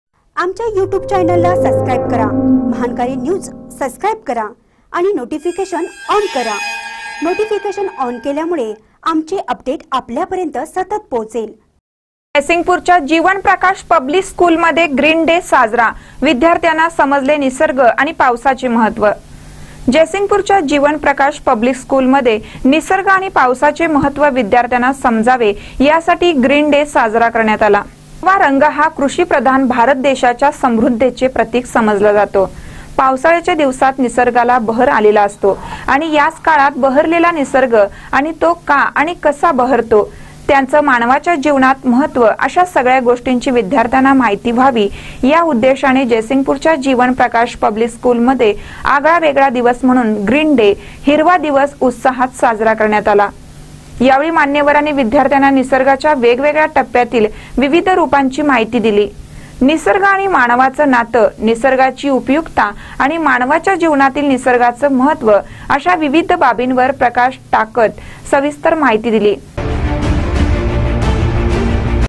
जयिंसगपूरच्या जीवन प्रकाश पब्लिक स्कूल मध्ये ग़्रीन डे साजरा
जयिंसगपूरच्या जीवन प्रकाश पब्लिक स्कूल मध्ये ग़्रीन डे साजरा : विद्यार्थ्याना समजले निसर्ग आणि पावसाचे महत्व